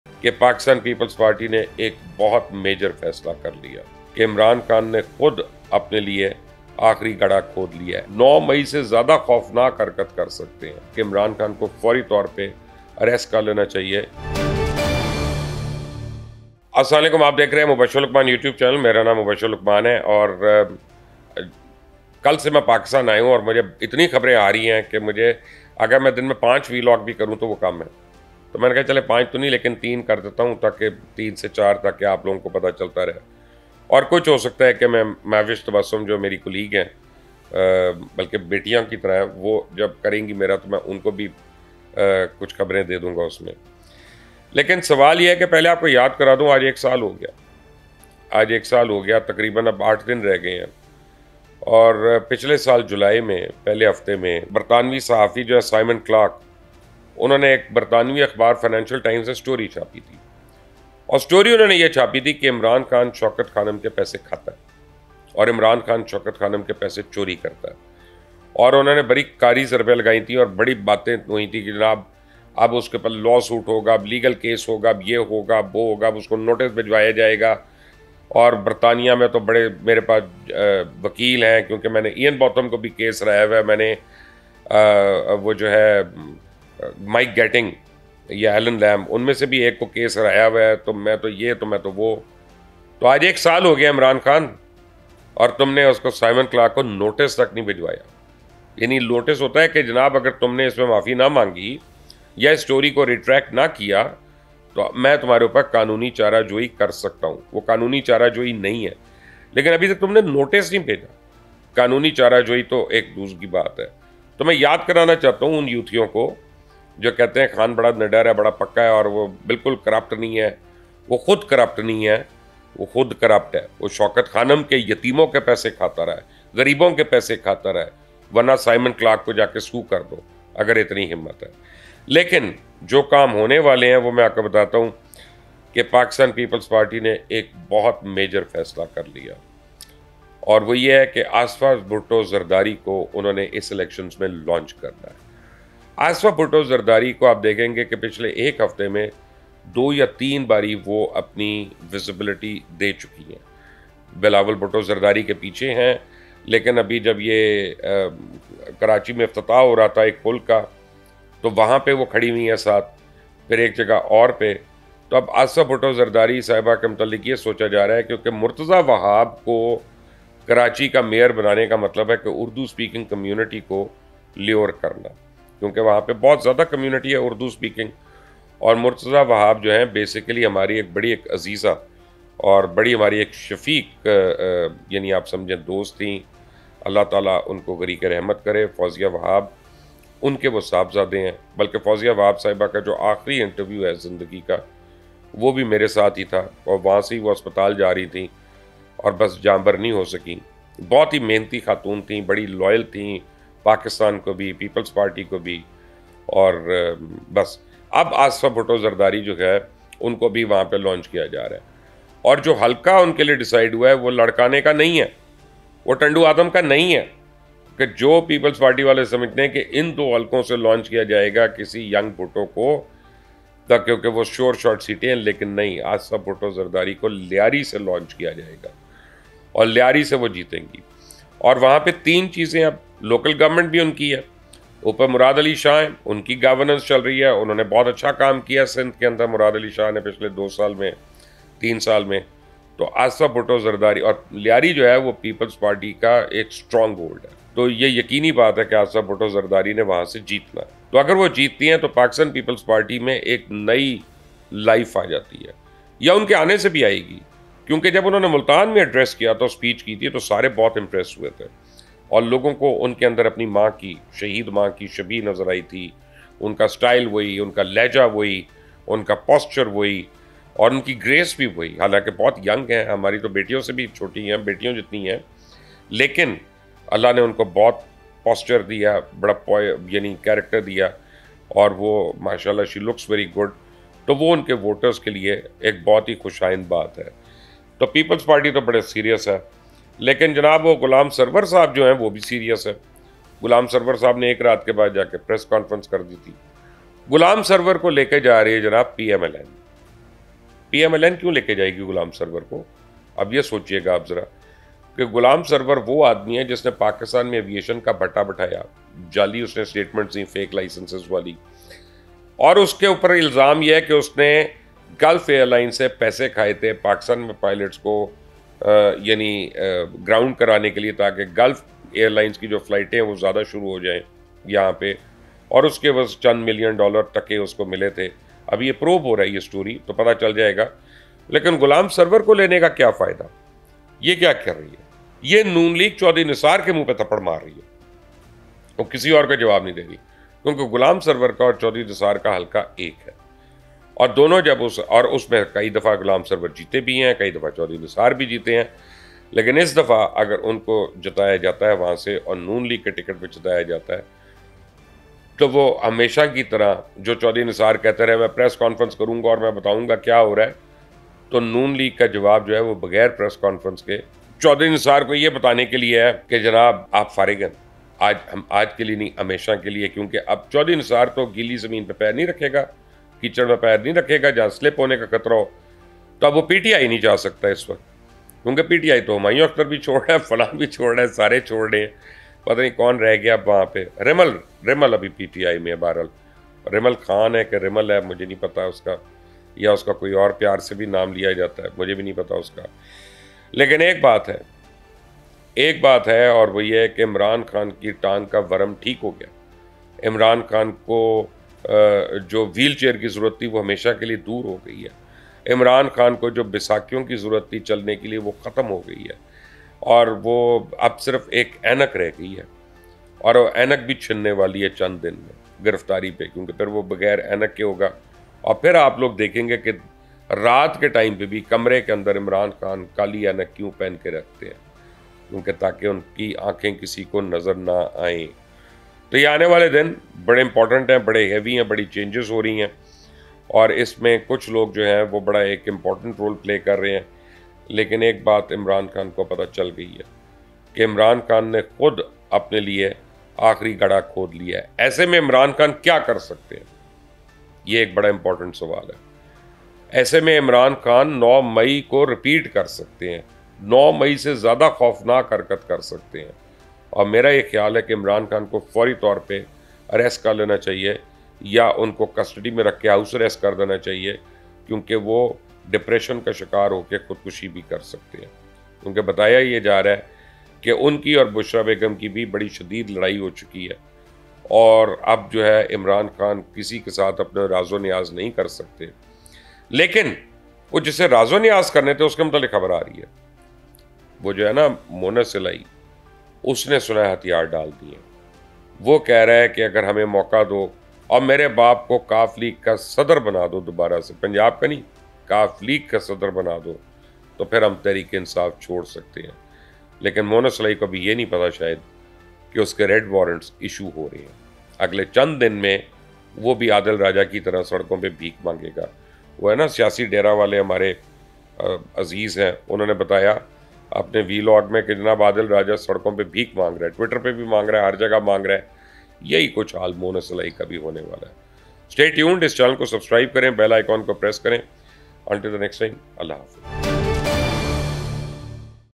Breaking news! Imran Khan stole money from Shaukat Khanum Hospital!
خوفناک سازش ، خودکشی کا خطرہ، راز دار بھاگ گیا
شوکت خانم کا پیسہ ہڑپ،کرپشن کا پول کھل گیا
 آصفہ کی لانچنگ ،آخری گڑھا کھد گیا۔
 اہم انکشافات
A famous investigative current affair talk show hosted by renowned Anchorperson Mubasher Lucman.